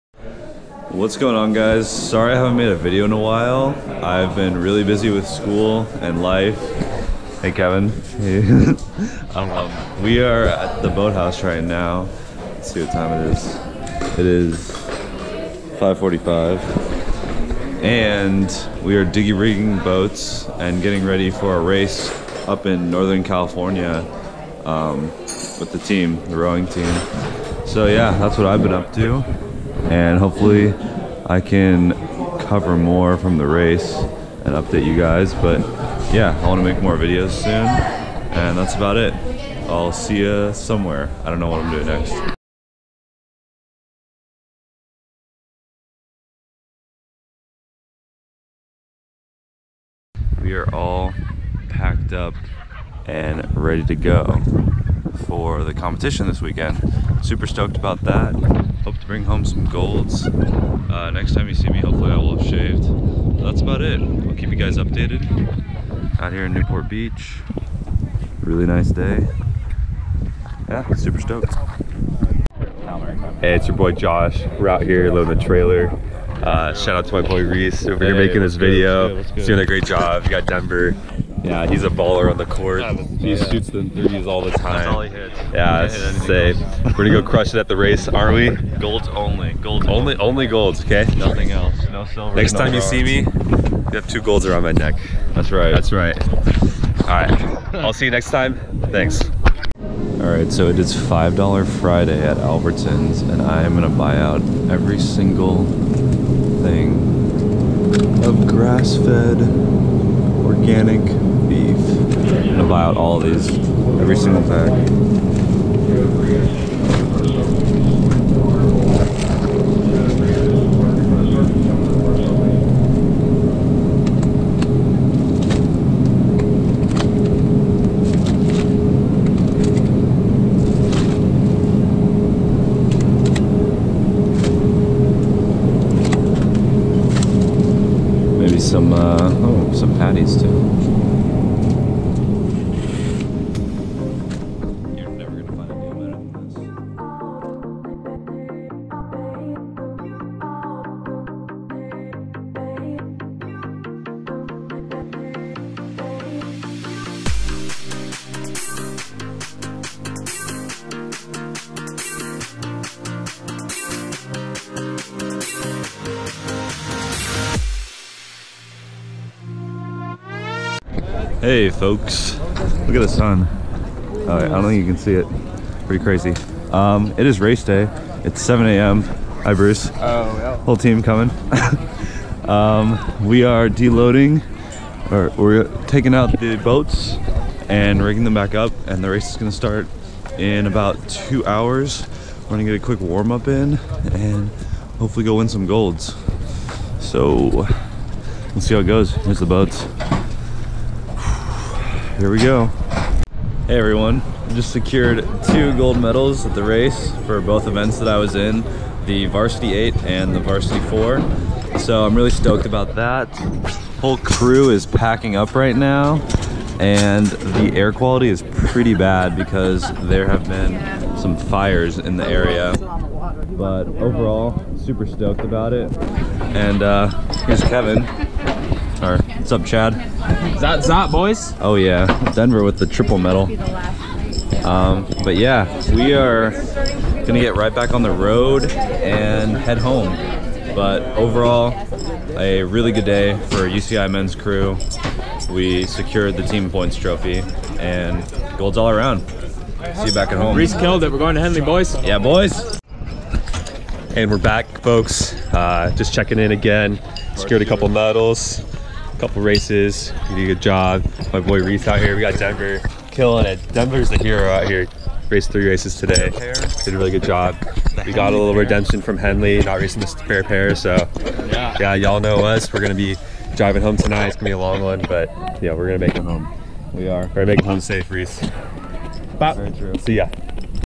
Head of the Lagoon Race Vlog
The big boys of uci men's crew take on head of the lagoon for a surprising ending you will not expect!